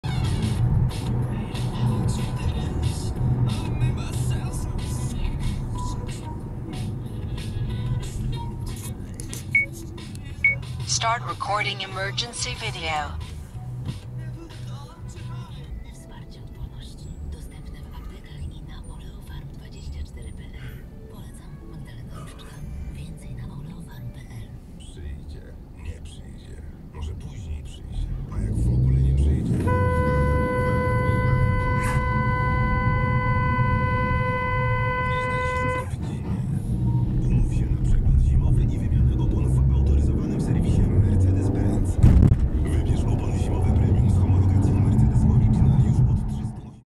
Czerwone i nieogarnięty motorniczy
Niestety standard jakim jest dodanie gazu jak widzimy żółte zamiast hamować oraz motorniczy który nie ogarnął sygnalizatora ...

00:00 czerwone światło
00:15 nieogarnięty motorniczy

Rejestrator: 70mai A800